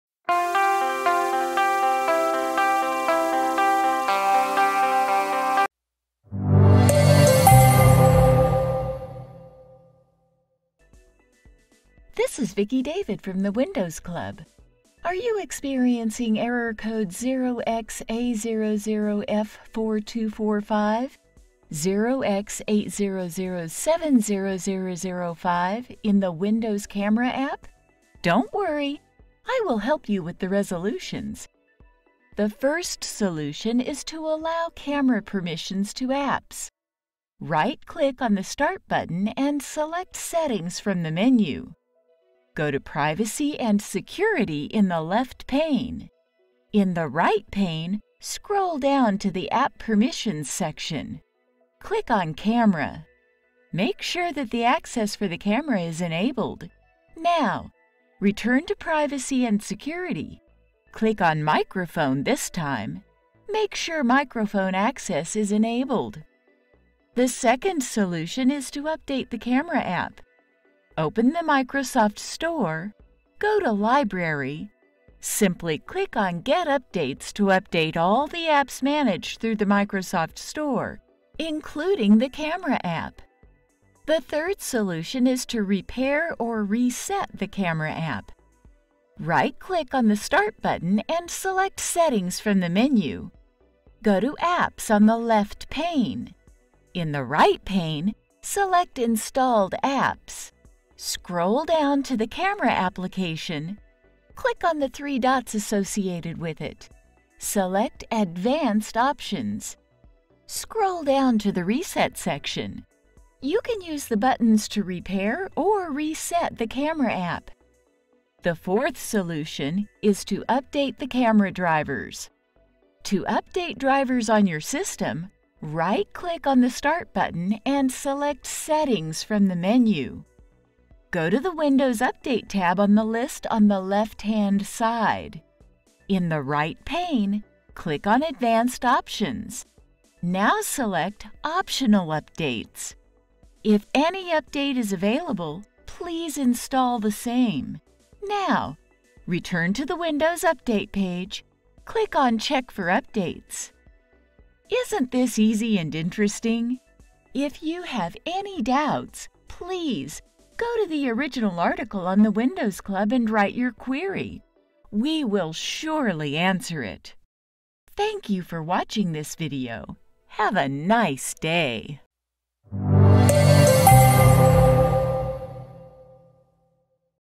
Fix 0xA00F4245 (0x80070005) Camera Error on Windows 11/10
Are you experiencing error code 0xA00F4245 (0x80070005) in the Windows Camera app? Several Windows users have reported getting this error code 0xA00F4245 (0x80070005) when they open their Camera app.

If you are experiencing error code 0xA00F4245 (0x80070005) in the Camera app on Windows 11/10, you can use the following fixes to resolve it:

1] Allow camera permission to apps.

2] Update the Camera app.

3] Repair/Reset Windows Camera.

4] Reinstall your camera/webcam driver.

5] Reinstall chipset drivers.

Timecodes:
0:00 Intro
0:33 Allow Camera permissions to apps
1:09 Update Camera app
1:27 Repair or Reset camera app
2:00 Update the Camera drivers